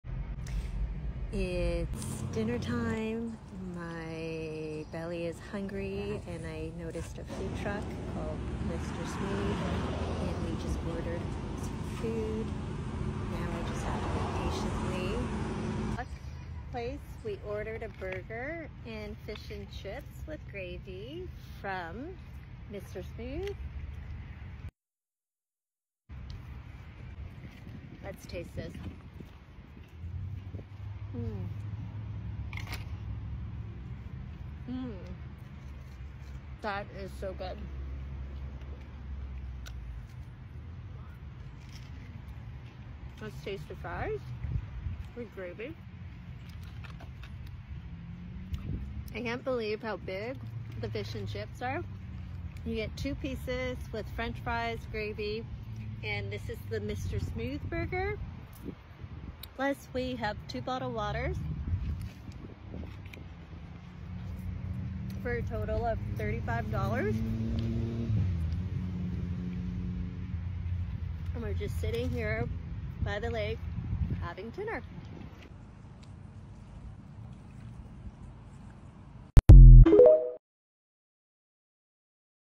Why nobody cooks in the summertime
During the summer, you experience a strong desire to break free and embark on exciting adventures. It's the perfect time to discover new dining spots, like Mr Smooth Food Truck and Catering. Their mouthwatering dishes are not only quick and delicious but also accompanied by exceptional service.

On our visit, we indulged in 2 delectable servings of fish & chips, complemented by a juicy Mr Smooth hamburger. To quench our thirst, we also enjoyed 2 refreshing bottles of water. All of this came at the affordable price of $35. If you find yourself in Halifax, be sure to visit one of their various locations and treat yourself to an unforgettable  experience.

Easy quick dinner supporting our local mrsmoothfoodtruckandcatering summereats foodie foodtruck foodreview #10 out of 10 with great service